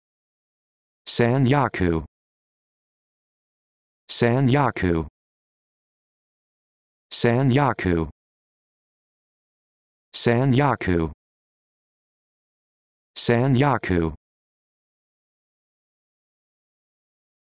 How to Pronounce Sanyaku
This video shows you how to pronounce Sanyaku